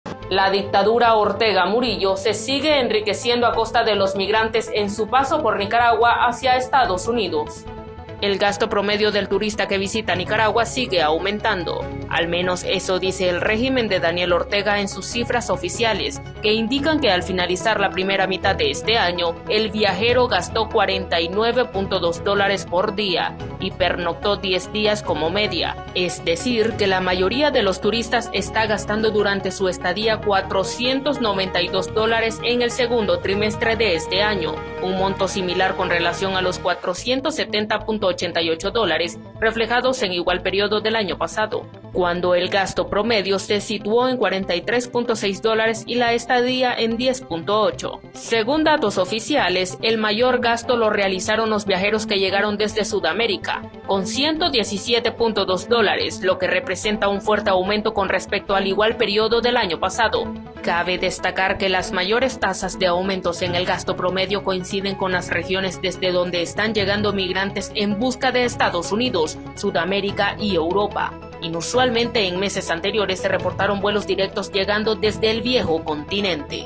Nicaragua: régimen lucrativo con migrantes y turismo en auge
El gobierno de Nicaragua, según expertos, ha establecido una lucrativa red en torno al tránsito de migrantes, facilitando vuelos, visados, transporte y estadías, y cobrando impuestos y tasas adicionales. Entre enero y octubre de 2023, el gobierno habría ingresado unos US$65,9 millones solo en tasas a migrantes, y en 2024 ya habría acumulado US$8,4 millones más. Los analistas creen que las ganancias reales para el gobierno son aún mayores, con cada migrante pagando entre US$10,000 y US$15,000 por la ruta completa desde su origen hasta la frontera con EE.UU.